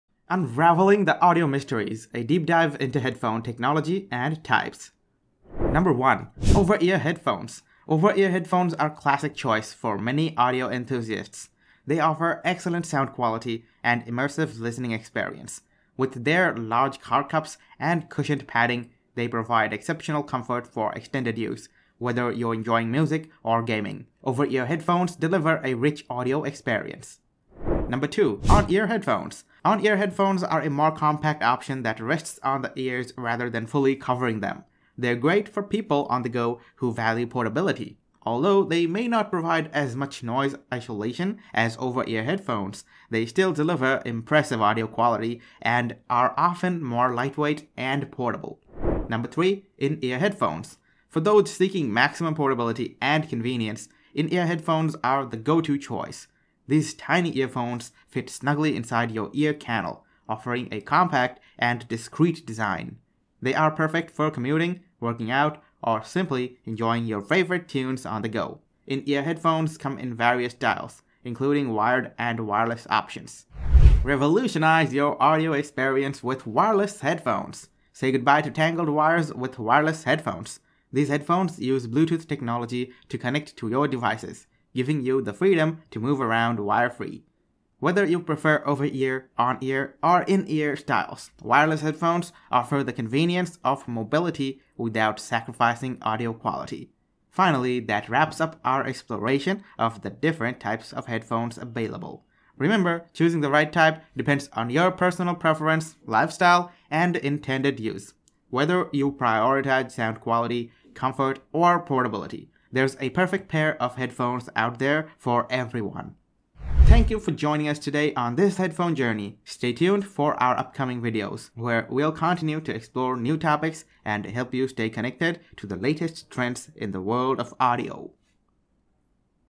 Types of Headphone
Unraveling the Audio Mysteries: A Deep Dive into Headphone Technology and Types!

1: Over-Ear Headphones

Over-ear headphones are a classic choice for many audio enthusiasts. They offer excellent sound quality and immersive listening experience. With their large ear cups and cushioned padding, they provide exceptional comfort for extended use. Whether you're enjoying music or gaming, over-ear headphones deliver a rich audio experience.

2: On-Ear Headphones

On-ear headphones are a more compact option that rests on the ears rather than fully covering them. They're great for people on the go who value portability. Although they may not provide as much noise isolation as over-ear headphones, they still deliver impressive audio quality and are often more lightweight and portable.

3: In-Ear Earphones

For those seeking maximum portability and convenience, in-ear earphones are the go-to choice. These tiny earphones fit snugly inside your ear canal, offering a compact and discreet design. They are perfect for commuting, working out, or simply enjoying your favorite tunes on the go. In-ear earphones come in various styles, including wired and wireless options.

[Next-hook line]
[Revolutionize Your Audio Experience with Wireless Headphones!]

Say goodbye to tangled wires with wireless headphones! These headphones use Bluetooth technology to connect to your devices, giving you the freedom to move around wire-free. Whether you prefer over-ear, on-ear, or in-ear styles, wireless headphones offer the convenience of mobility without sacrificing audio quality.

Finally:
That wraps up our exploration of the different types of headphones available. Remember, choosing the right type depends on your personal preferences, lifestyle, and intended use. Whether you prioritize sound quality, comfort, or portability, there's a perfect pair of headphones out there for everyone.